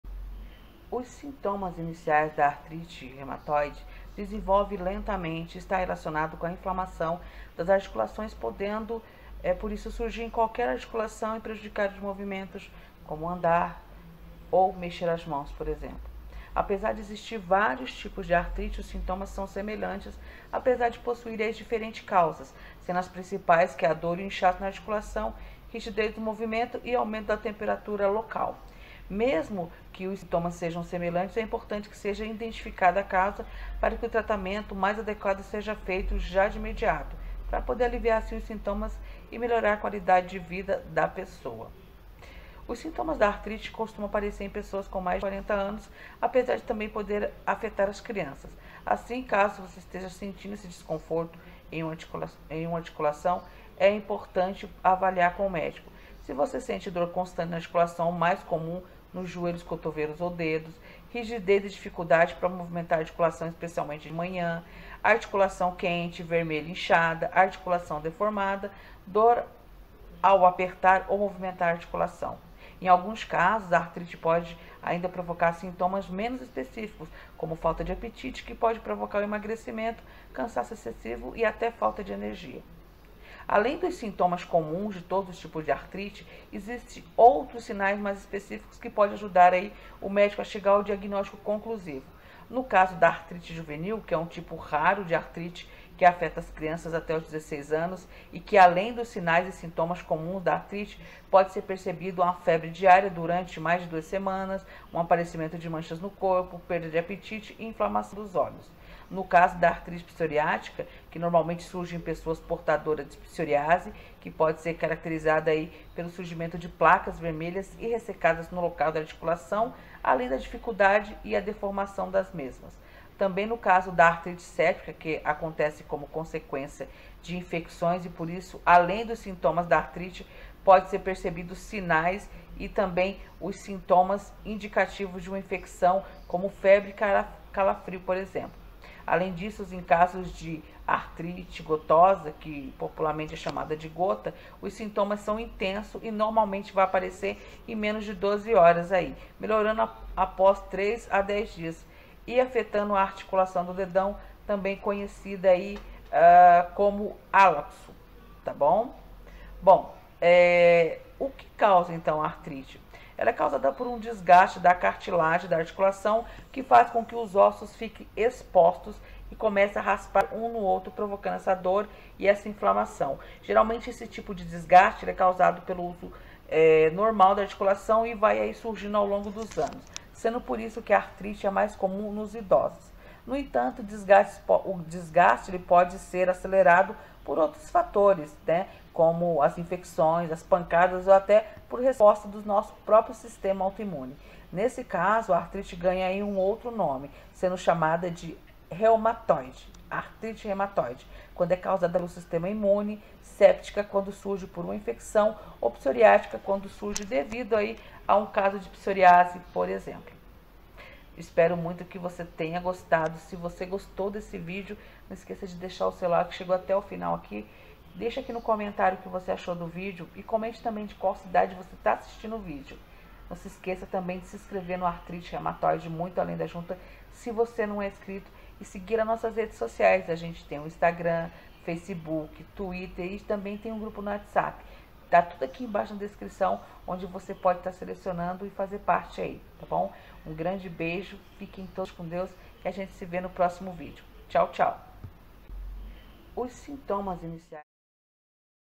sintomas iniciais da artrite reumatoide ou artrite
a dor surge tipicamente em repouso e melhora em caso de movimento. Em inflamações graves, pode haver dores noturnas na articulação,
que fazem a pessoa acordar.
Uma artrite pode aparecer subitamente (aguda) ou no decurso de uma inflamação lenta (crónica) e prevalecer por muito tempo.

CONTA POUPANÇA
AGÊNCIA: 0314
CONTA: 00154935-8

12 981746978